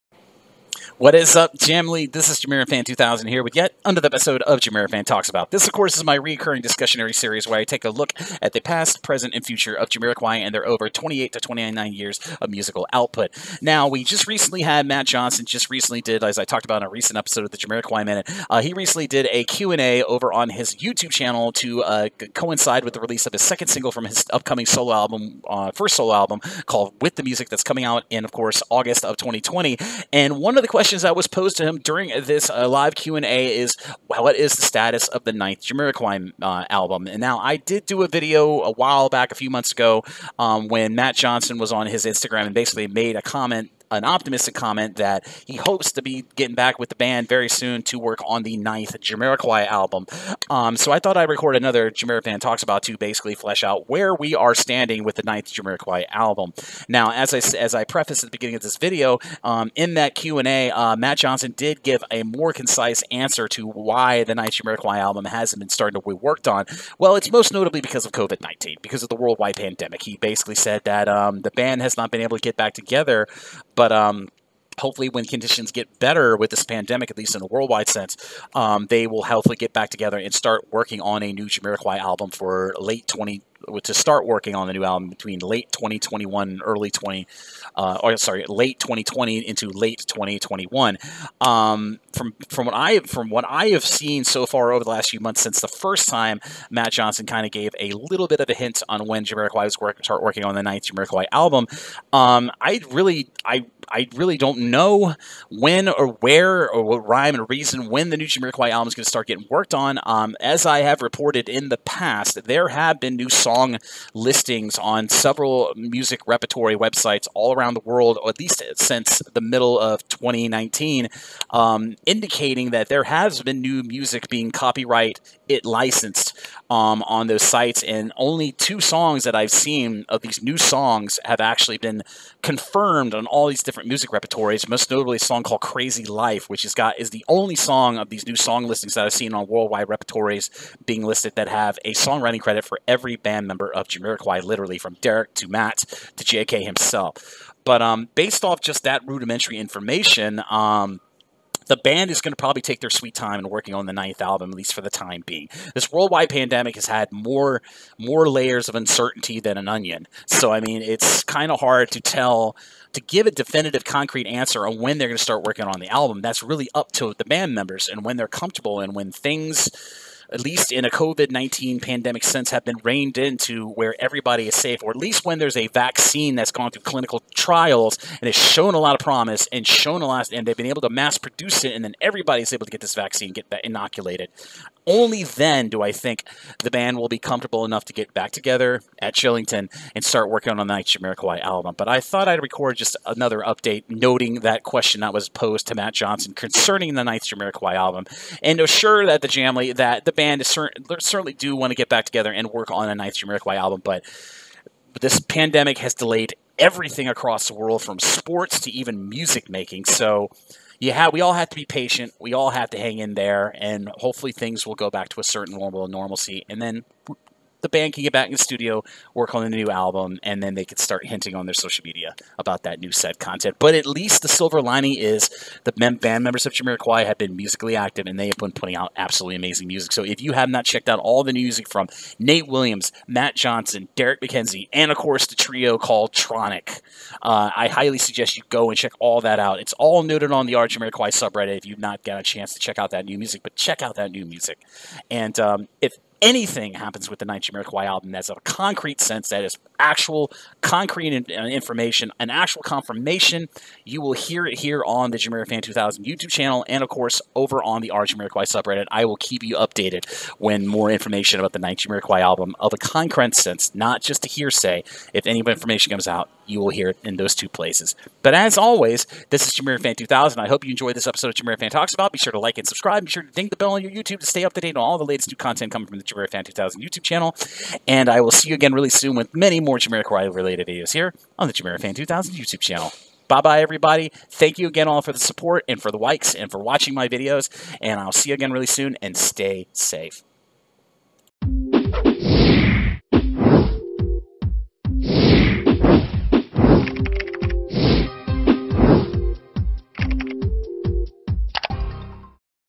'JamiroFan Talks About..' | Latest Status Of 9th Jami Album!
Hello,

Welcome to another new episode of 'JamiroFan Talks About', my recurring discussionary series looking at the past/present & future of Jamiroquai!

In today's episode I look at the current status of the 9th Jamiroquai Album in post Covid world.